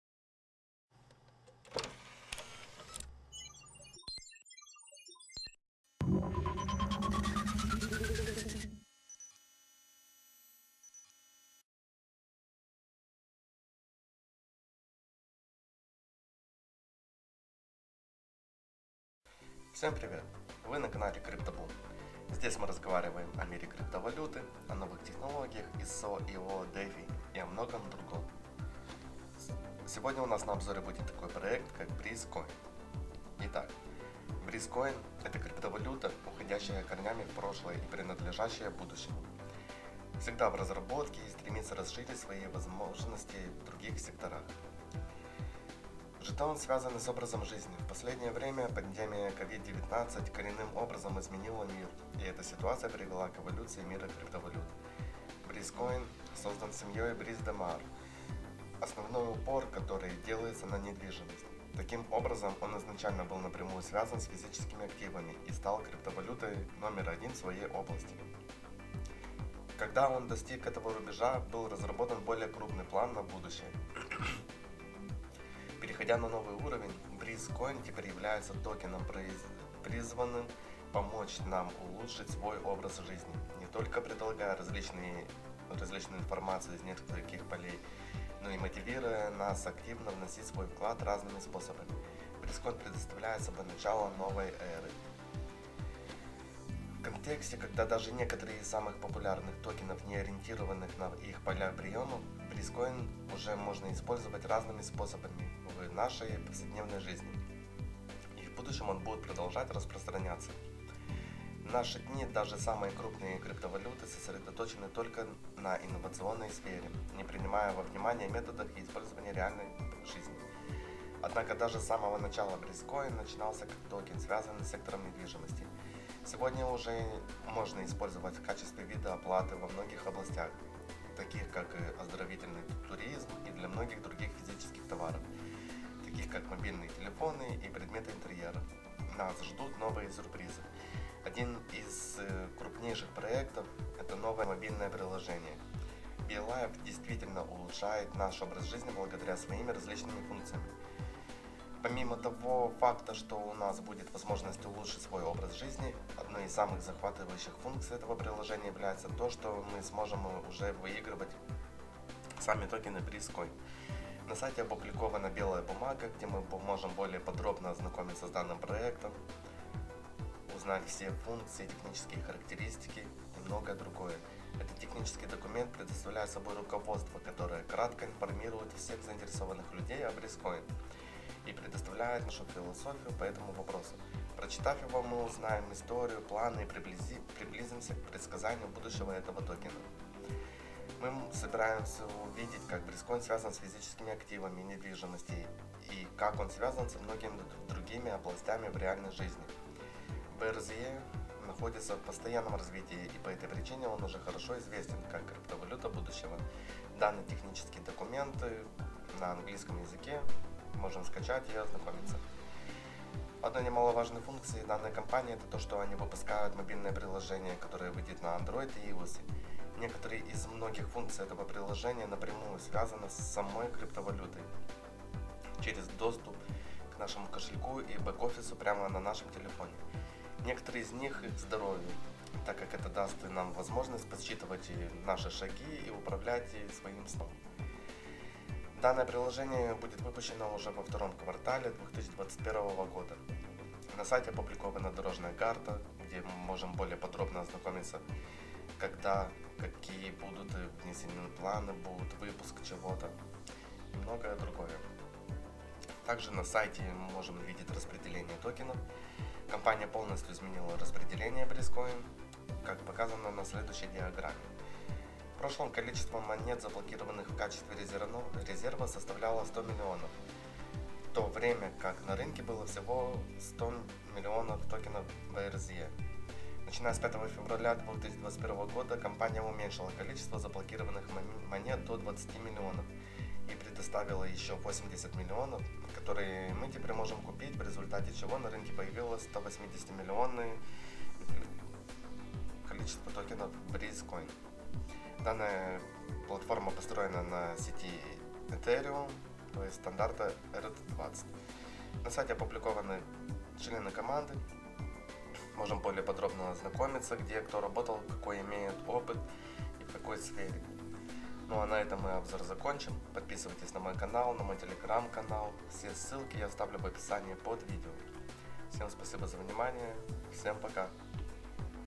Обзор проекта - Breezecoin
Breezecoin is a cryptocurrency well-rooted in the past, and belonging to the future
Always in development, and looking to extend into other sectors